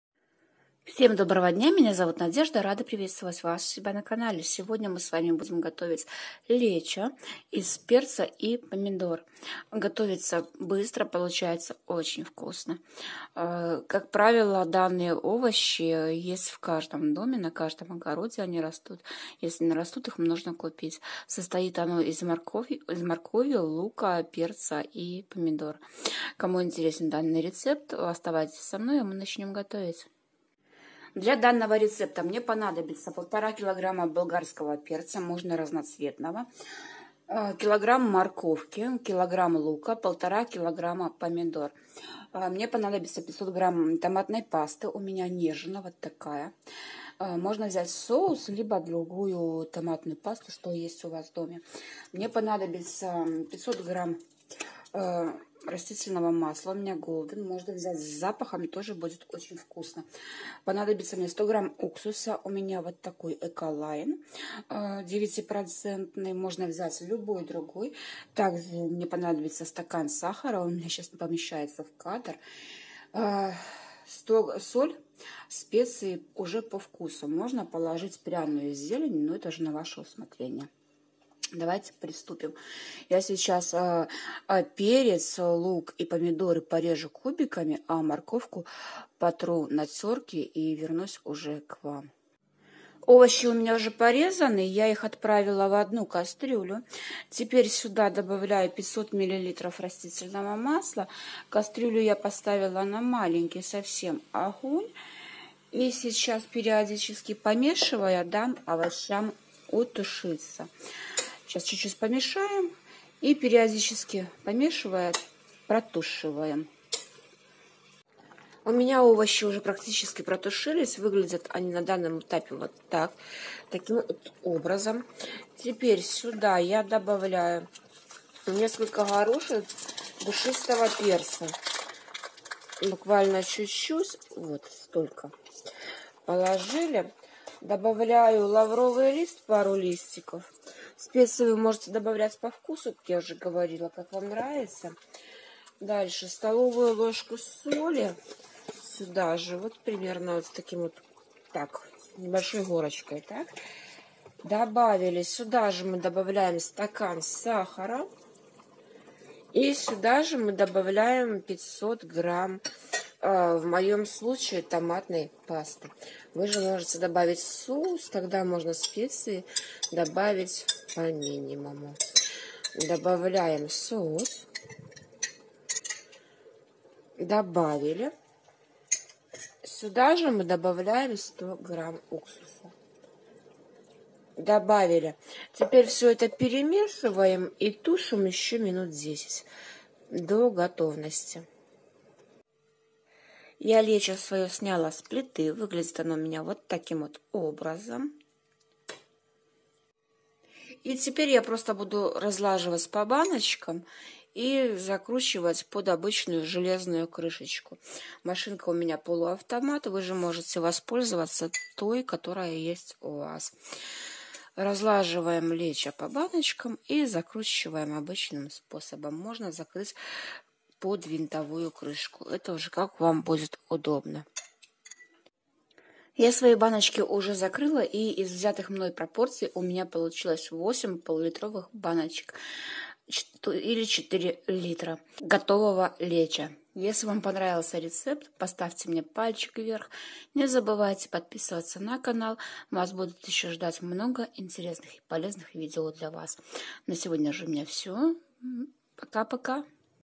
Лечо на зимнее хранение. Рецепт.
Всем доброго дня. Сегодня я покажу вам рецепт лечо на зимнее хранение. В основе состава находится болгарский перец томаты лук и морковь. Готовится не сложно. Результат вас приятно удивит.